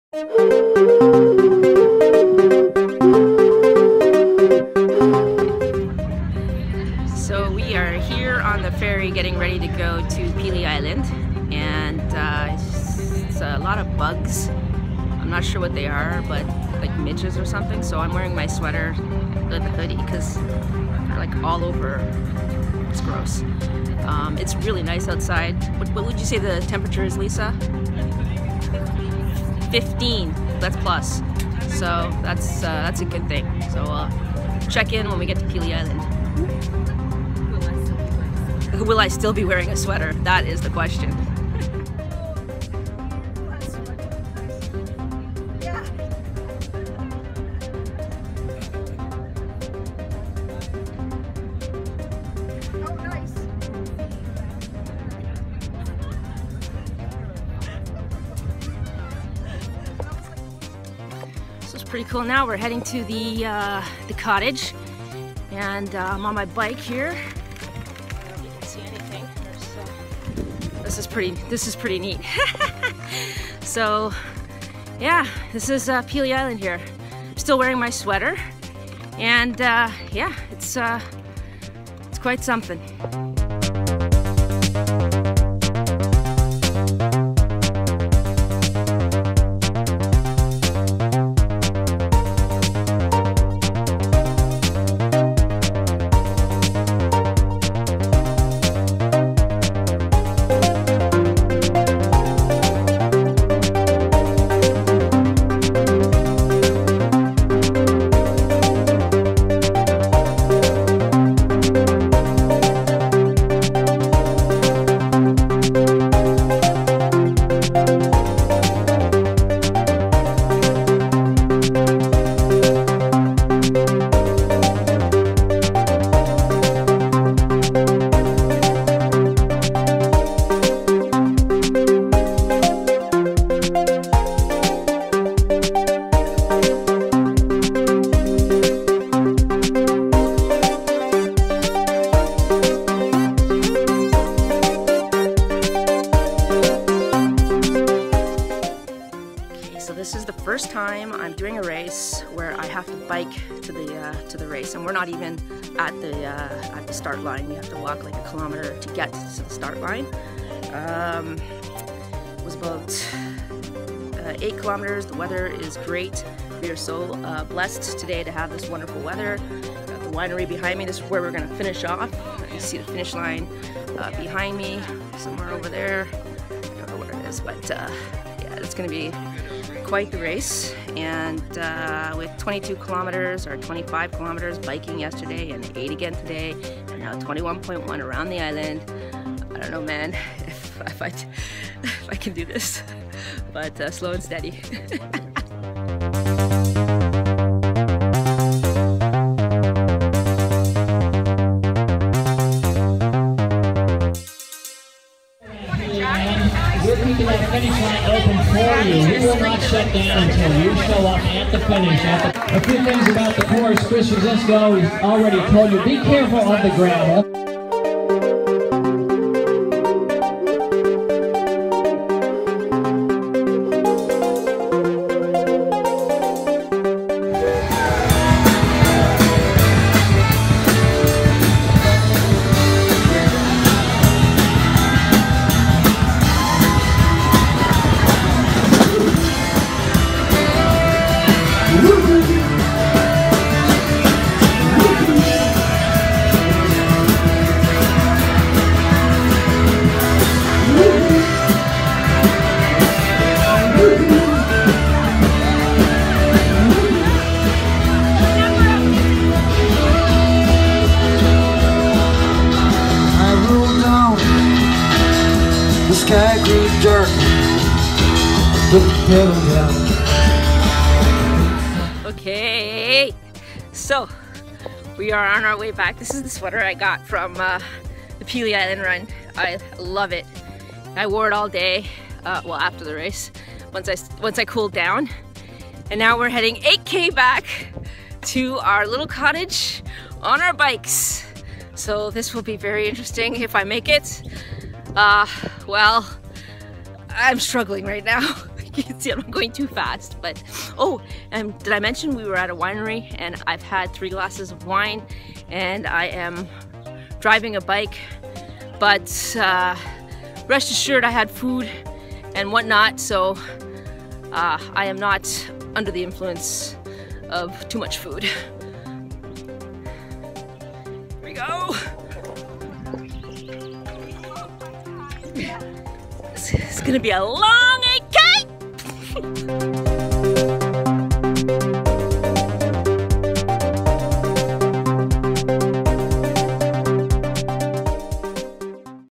Sweater Diaries — Pelee Island
Weekend of June 2 - 4, 2017 on Pelee Island featuring Puma Jamaica 🇯🇲 hoodie and a new one from Running Flat 1/2 marathon.